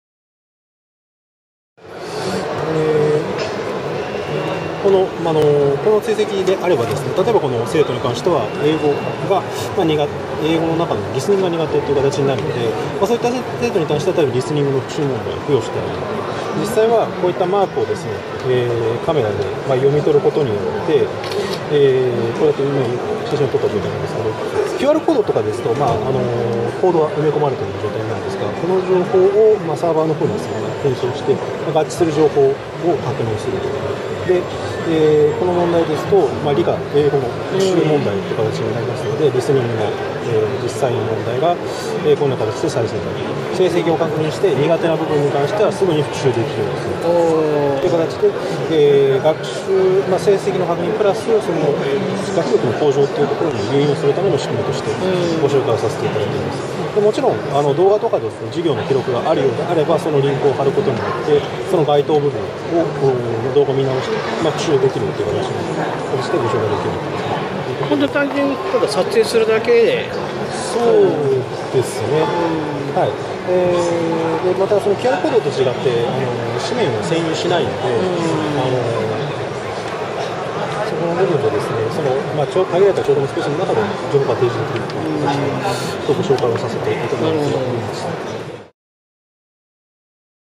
富士ゼロックスシステムサービス：デジタルリンク成績表 for iPad
富士ゼロックスシステムサービスが参考展示した「デジタルリンク成績表 for iPad」製品紹介